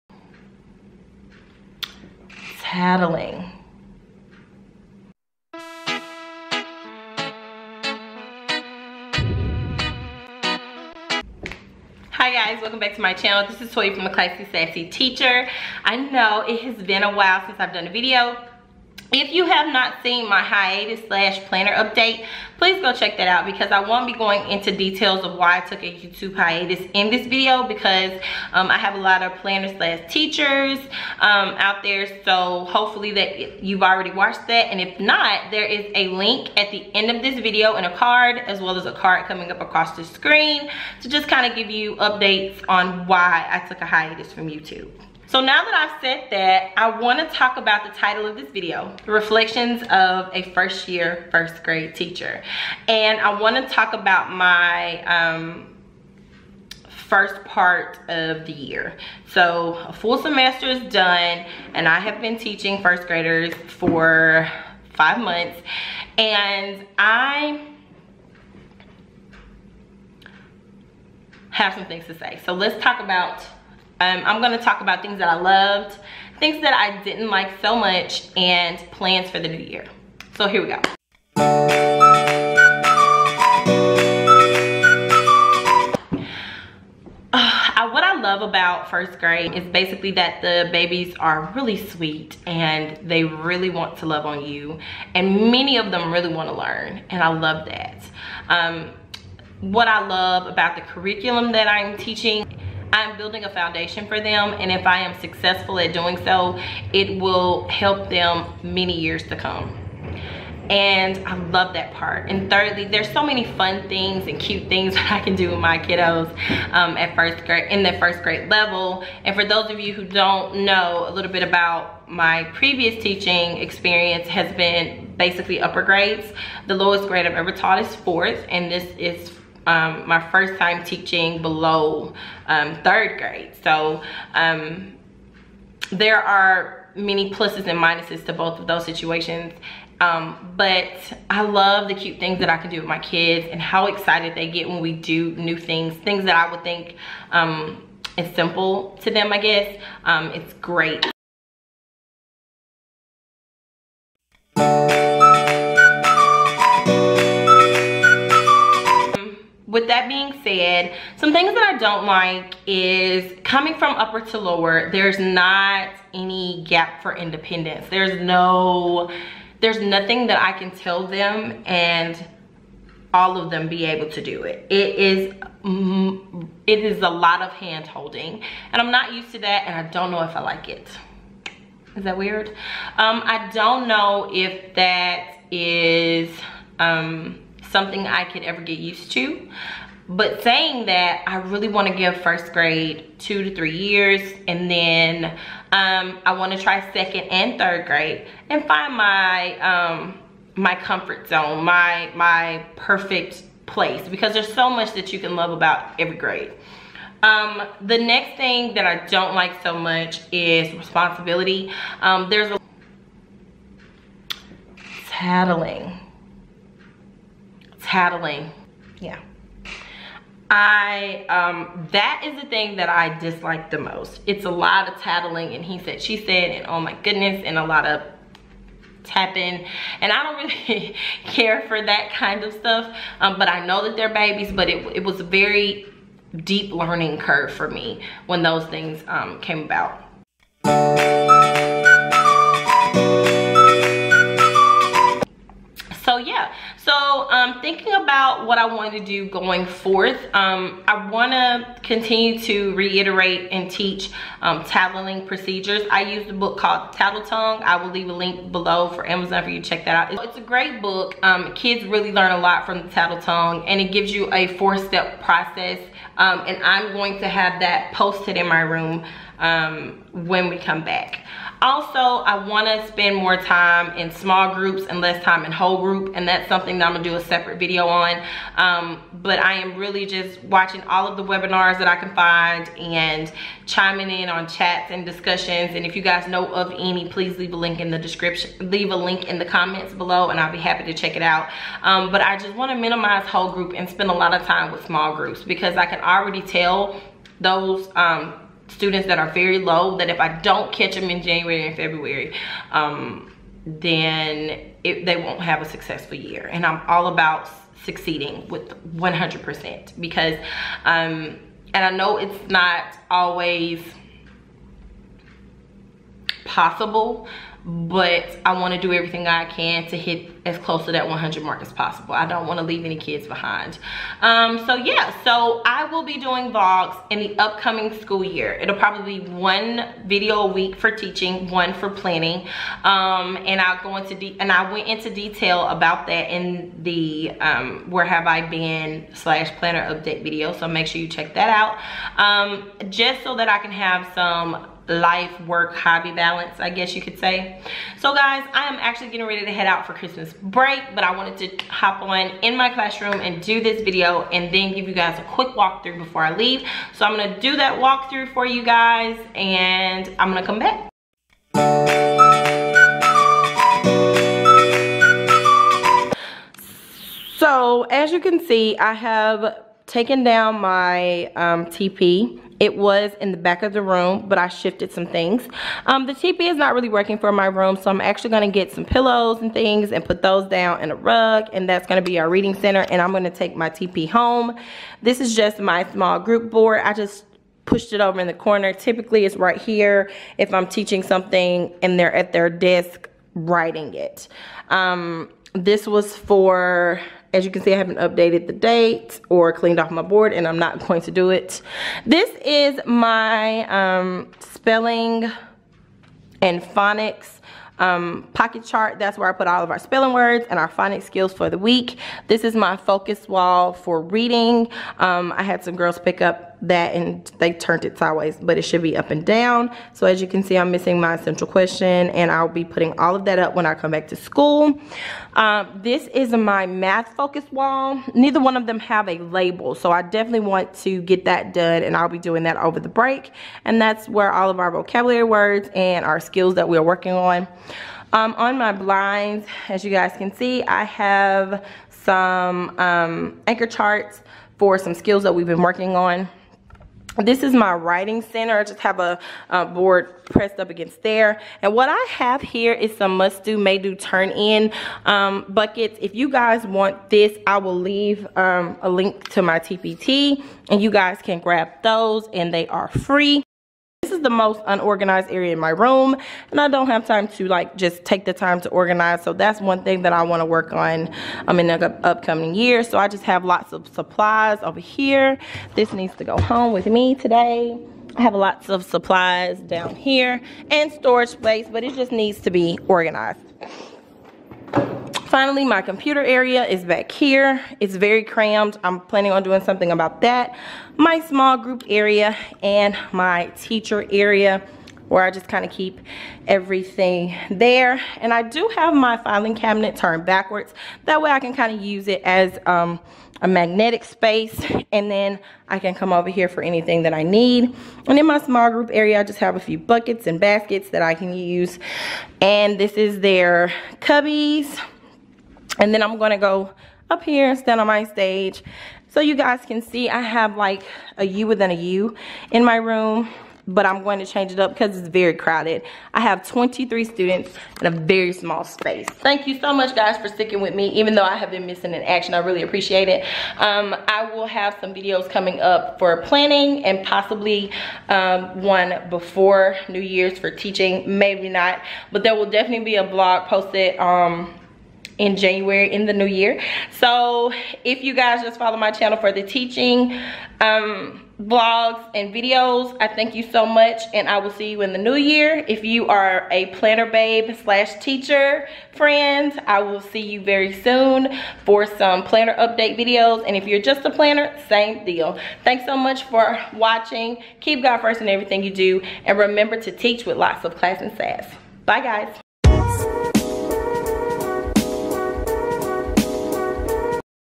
Confessions of a 1st grade Teacher: 1st Semester Teacher Reflection
Hi guys!!! Today's video is a reflection of my first half year teaching 1st graders. I will go into detail in a separate video of what worked and what didn't work in January. Let me know what you think! I missed you guys.

I  would love to hear from you guys.

Toya
theClassySassyLife